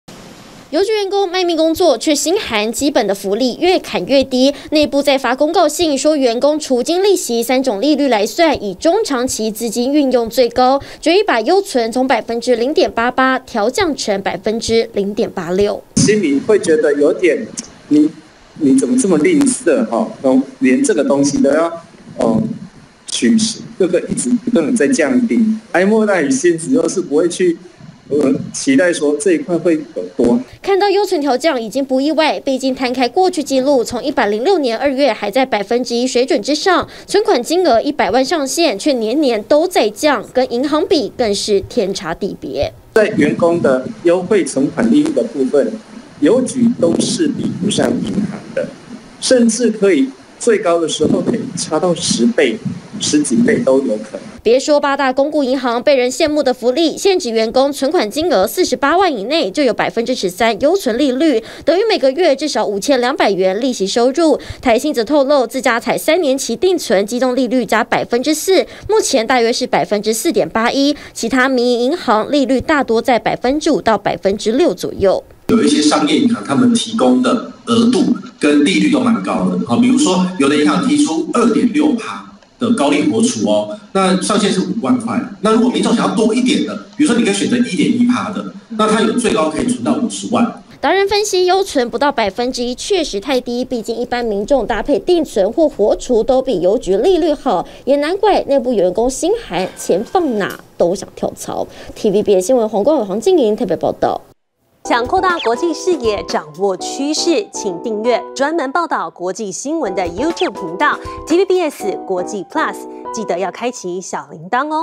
郵局員工優存降剩0.86% 產工:哀莫大於心死｜TVBS新聞
郵局員工有說不出，因為內部再公告說優存利率調降成0.86%，幾乎年年都在調低，產工砲轟員工已經哀莫大於心死，因為相比公股銀行，存款金額48萬元內，優存利率有13%，一般民眾搭配定存或活儲，也都至少有2%。